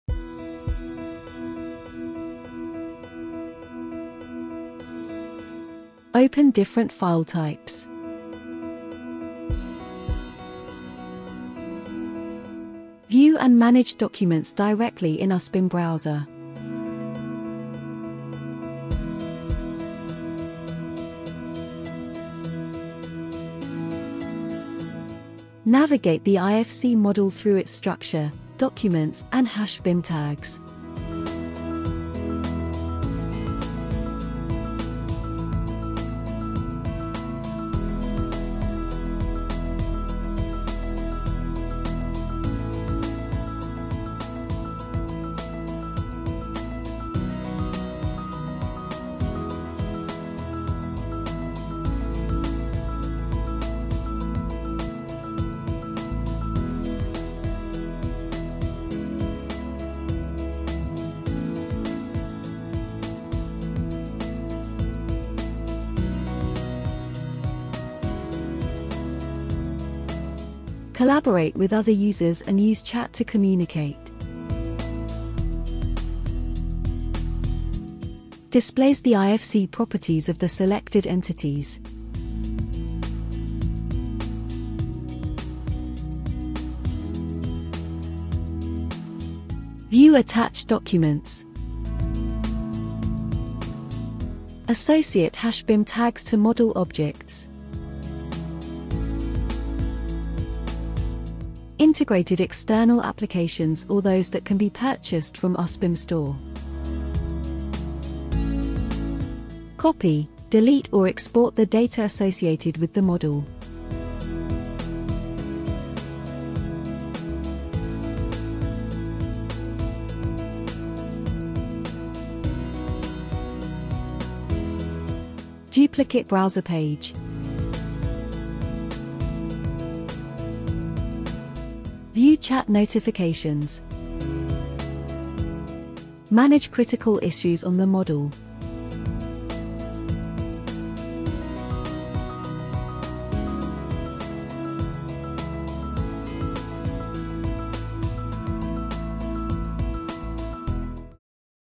usBIM Tutorial - usBIM.browser - ACCA software
How to view and manage BIM documents and models online
Need extra details regarding the usBIM cloud-integrated system?
See how to manage folders and documents with usBIM, the service that allows you to freely collaborate in real-time with your colleagues, share and manage construction and infrastructure projects, and work on-line with any device (pc, tablet, smartphone...) and from anywhere. Interested in finding out more?